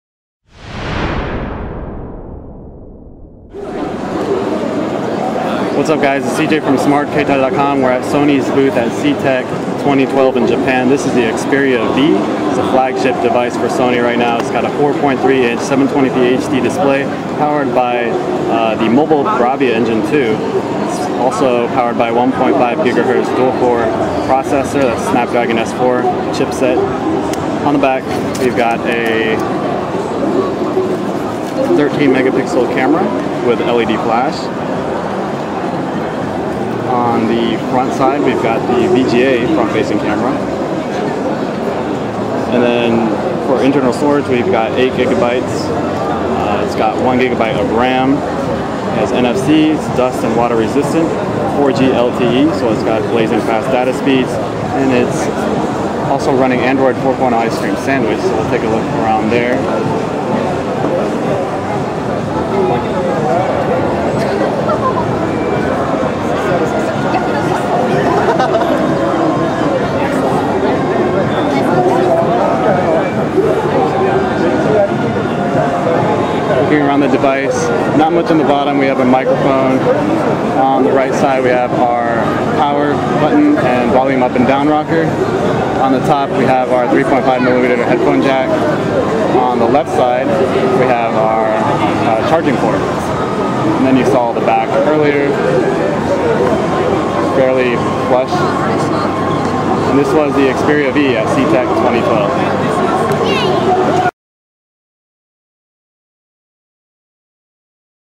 Sony Xperia V Android Smartphone Demo at CEATEC 2012 Japan
The Xperia V is very similar to the Xperia T, but it also has 4G LTE connectivity for lightning-fast data speeds. The device features a slightly smaller 4.3-inch HD 720p Reality display, 1.5 GHz Qualcomm MSM8960 dual core processor, 13 megapixel camera with Exmor R for mobile image sensor, 8GB of internal storage (expandable with microSD card), 1GB of RAM, PlayStation certification, MHL connectivity, and Android 4.0.4 Ice Cream Sandwich (update to Jelly Bean later). Sony is planning to release this one in Q4 of 2012. Full specs, picture gallery and video of the Xperia V posted after the jump.

Xperia V Specifications and Features:
- 4.3" HD 1280×720 Reality Display powered by the Mobile BRAVIA Engine 2 for razor sharp clarity
- 13 MP fast capture camera with full HD video recording and Exmor R for mobile image sensor.
- VGA front-facing camera
- 1.5GHz dual core Qualcomm MSM8960 Snapdragon processor for improved battery life, fast performance and ultra sharp graphics
- 4G LTE for the fastest data download speeds (in markets where technology is available)
- 8GB of internal storage (expandable via microSD), 1GB of RAM
- Easy connectivity with One-touch 'One-touch' enabled by NFC
- Clear audio+ for the highest level of sound quality
- PlayStation Certified
- Android version 4.0.4 Ice Cream Sandwich. Will be upgraded to Android version 4.1 (Jelly Bean) following launch. Detailed timing to follow
- Highest levels of dust and water resistance in a smartphone (IP55/57+) ensure protection from the effects of immersion up to 1m for 30 minutes
- MHL connectivity for viewing content on a big screen without the need for a charger
- Available in Black, Pink and White colours
- Launching globally Q4 2012